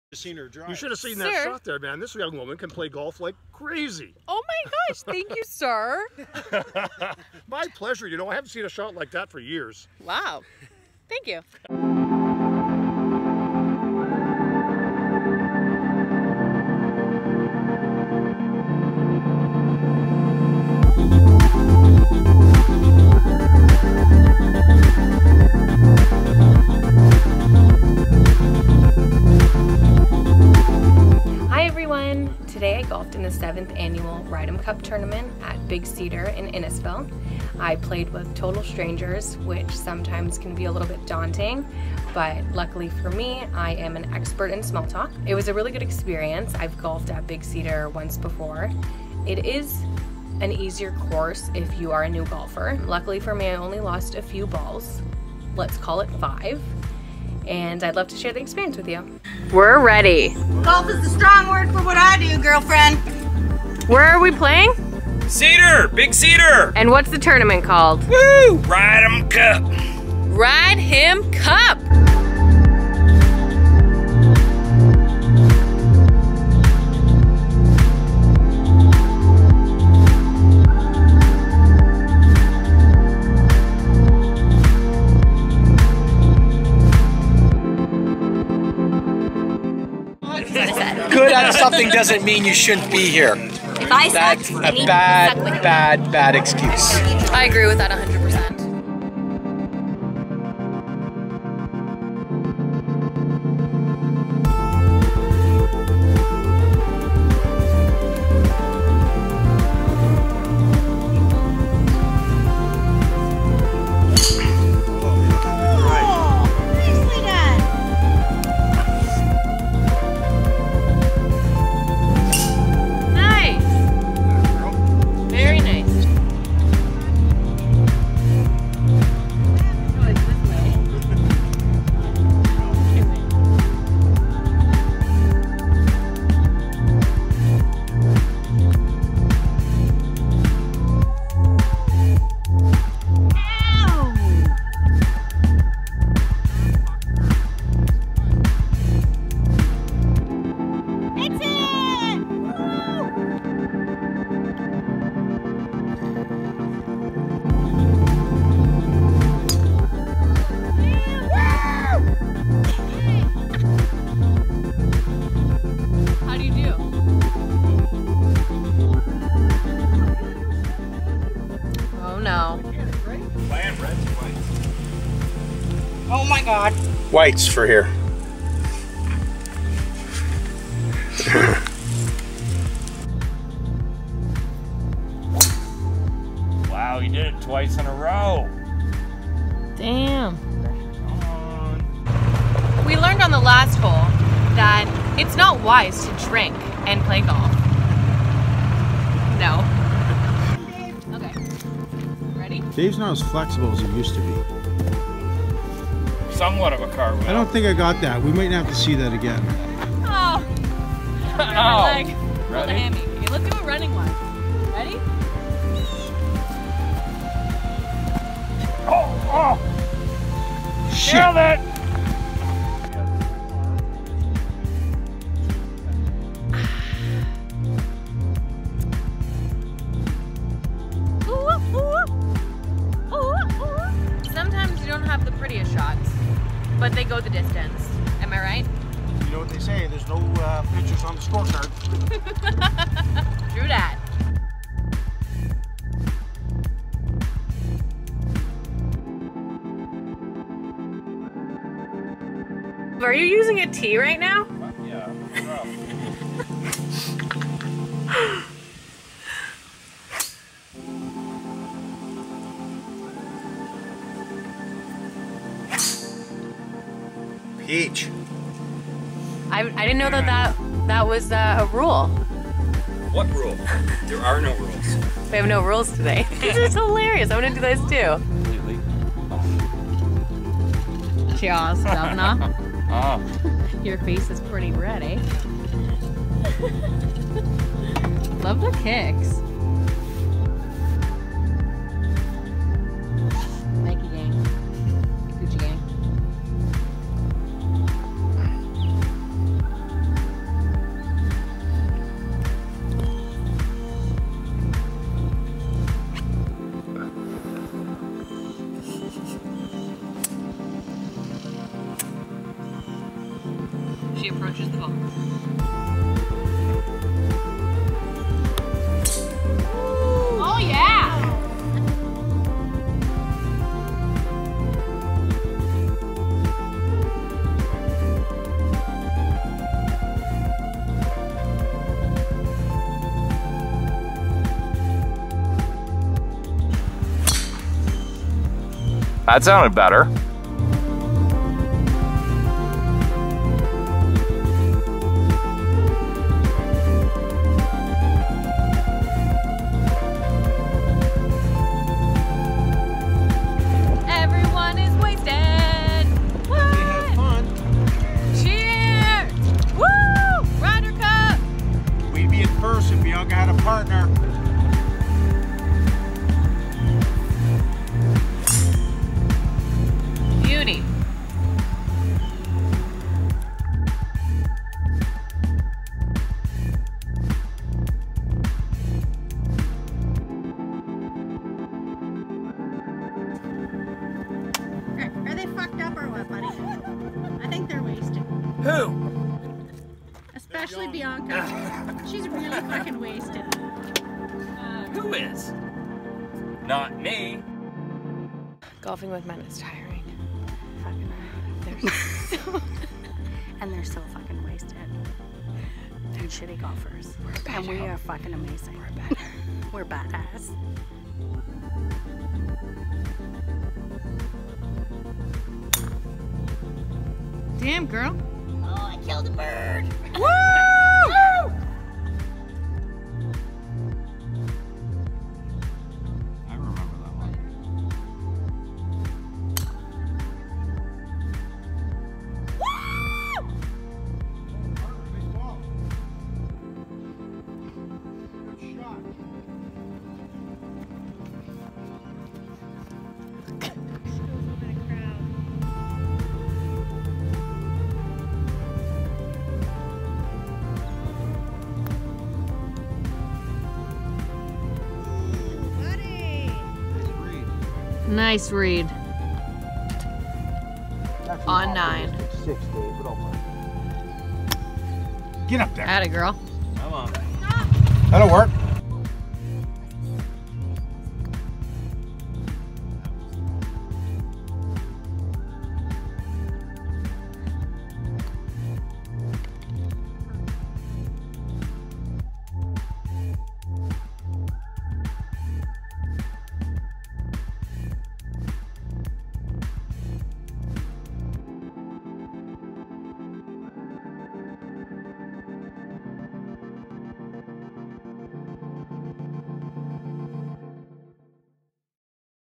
Golf gone WILD!
What a FUN day golfing at Big Cedar in the 2018 Ride Him Cup Tournament!  Can't wait for next year :)